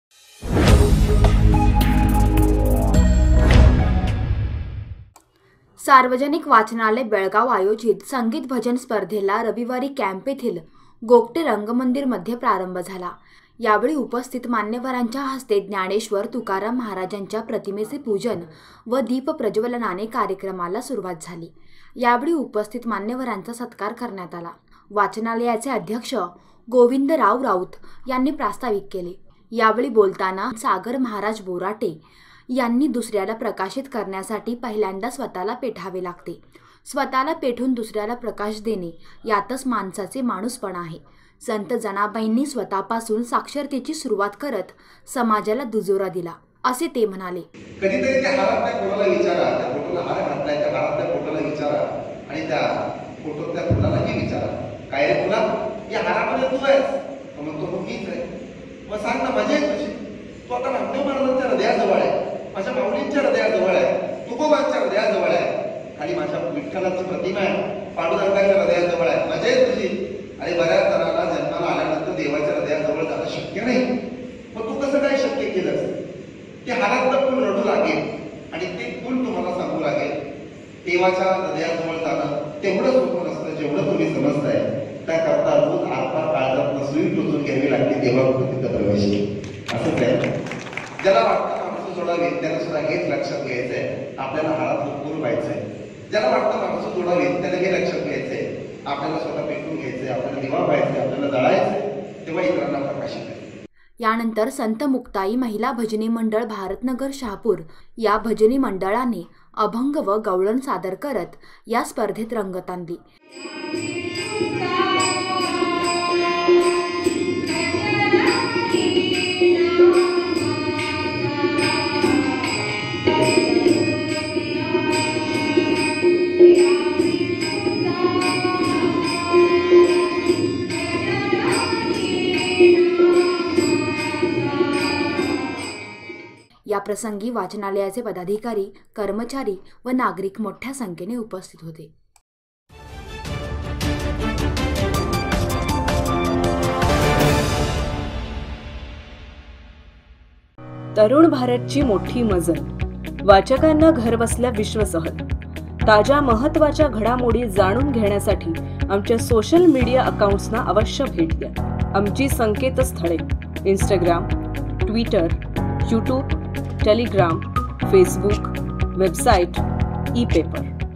सार्वजनिक वाचनालयाच्या भजन स्पर्धेला उत्साहात प्रारंभ | Belgaum News | 26-08-2019
सार्वजनिक वाचनालय बेळगाव आयोजित संगीत भजन स्पर्धेला रविवारी कॅम्प येथील गोगटे रंगमंदिरमध्ये प्रारंभ झाला. यावेळी उपस्थित उपस्थित मान्यवरांच्या हस्ते ज्ञानेश्वर, तुकाराम महाराजांच्या प्रतिमेचे पूजन व दिपप्रज्वलनाने कार्यक्रमाला सुरुवात झाली.
|Tarun Bharat Social Media | तरुण भारत | Sarvajanik vachnalay bhajan spardha |